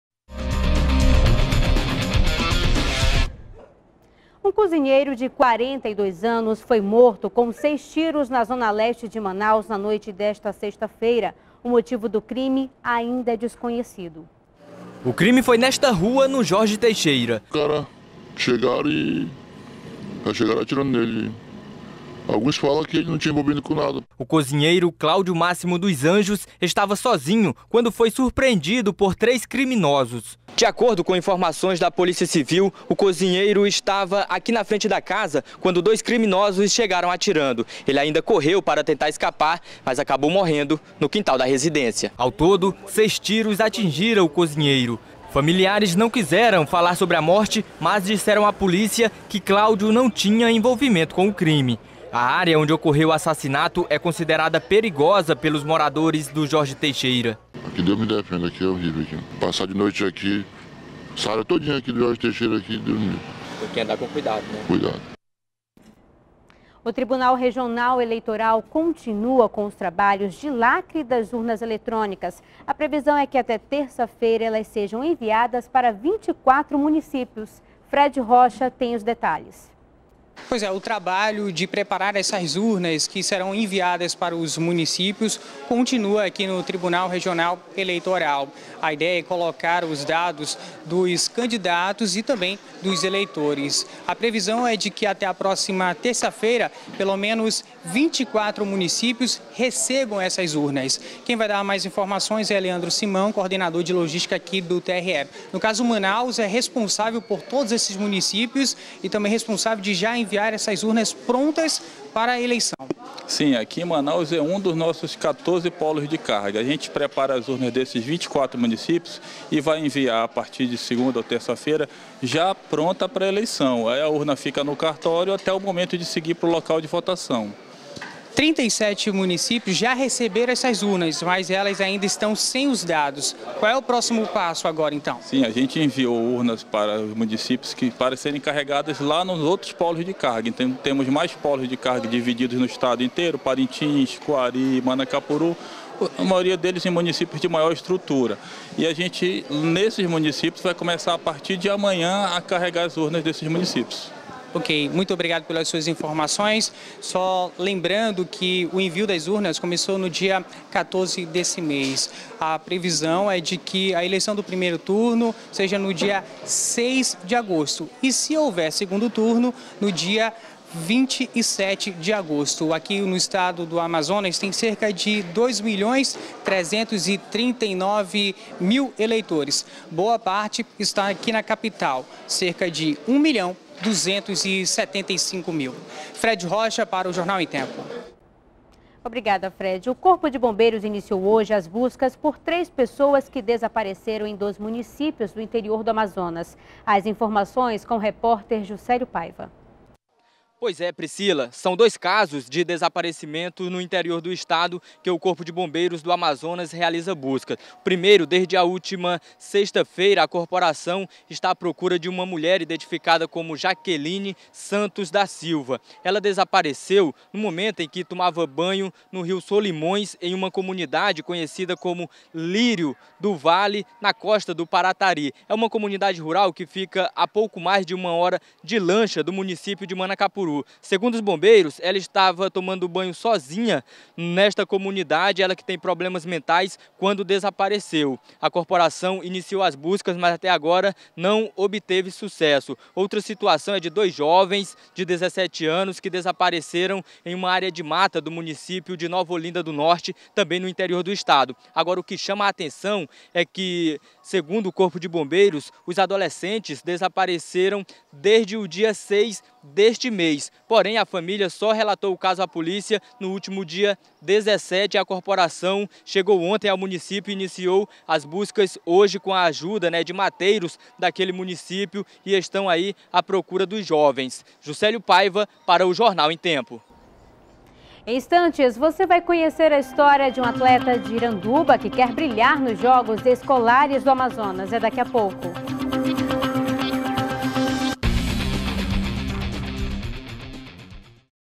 Cozinheiro de 42 anos é morto com seis tiros na zona leste de Manaus
02º BLOCO JORNAL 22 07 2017 - Cozinheiro de 42 anos é morto com seis tiros na zona leste de Manaus